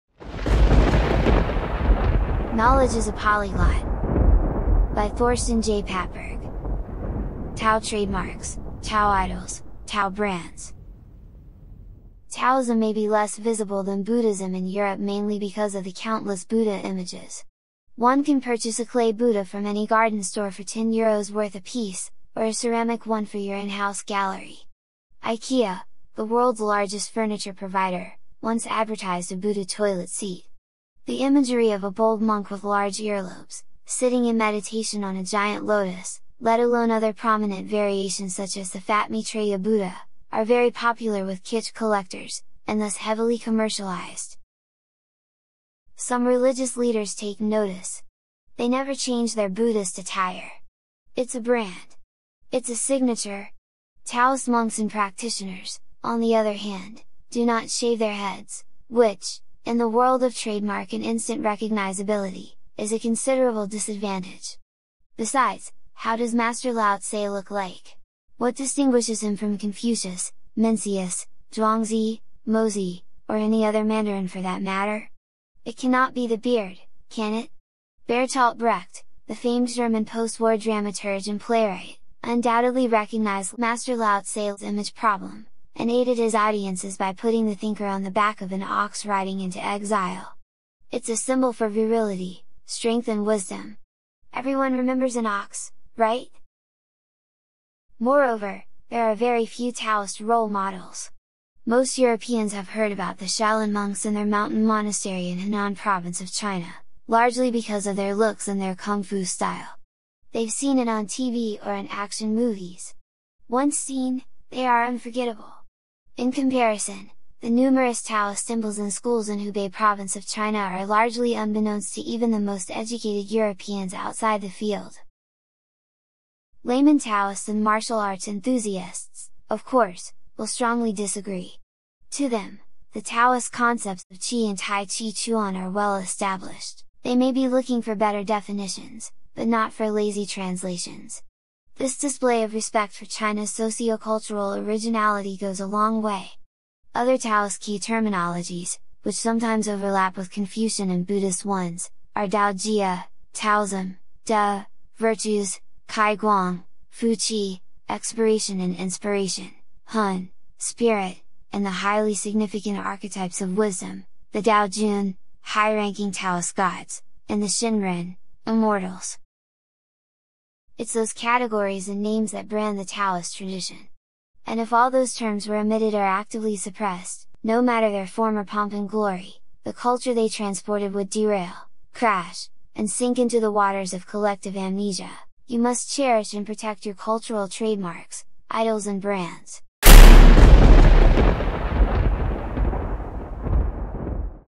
Knowledge Is A Polyglot - TAO Trademarks, Idols & Brands
Taoism may be less “visible” than Buddhism in Europe mainly because of the countless Buddha images. One can purchase a clay Buddha from any garden store for 10 Euros’ worth a piece; or a ceramic one for your in-house gallery. IKEA, the world’s largest furniture provider, once advertised a Buddha toilet seat. The imagery of a bold monk with large earlobes, sitting in meditation on a giant lotus, let alone other prominent variations such as the fat Maitreya Buddha, are very popular with kitsch collectors, and thus heavily commercialized.

Pattberg, Thorsten J. (2021), Knowledge Is A Polyglot - How to Translate Chinese Cultural Key Terminologies, LoD Press, New York